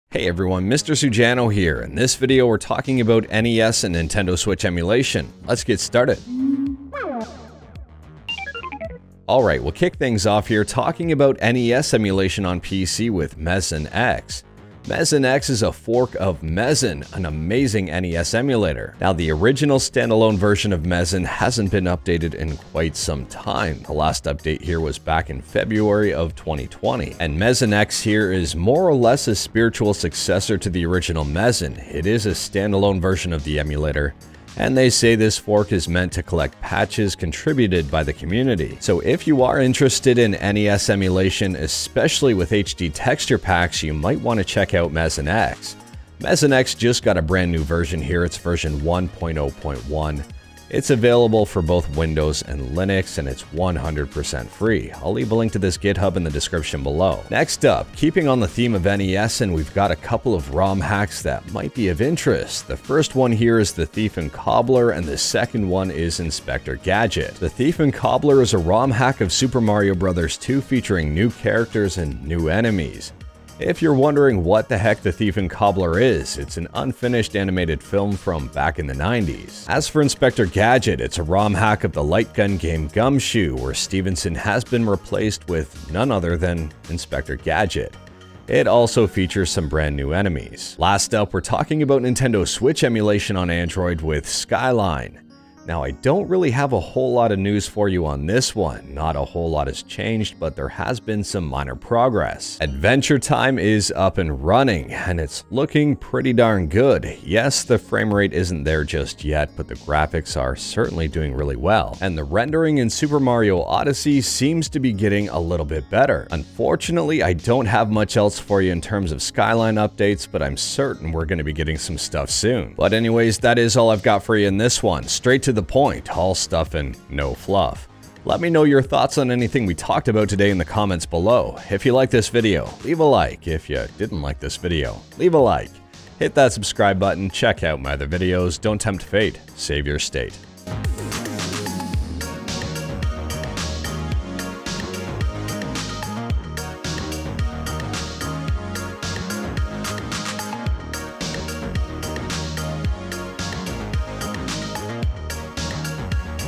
NES Emulator gets a surprise release and Nintendo Switch Emulation on Android Updates
Hey all! In this video we go over the latest and greatest in Emulation Updates, news and tips! Emulators covered in this video: NES (Mesen), Nintendo Switch (Skyline)

0:00 Mesen X gets a new release
0:57 New NES ROM hacks
1:30 Skyline Emulator Updates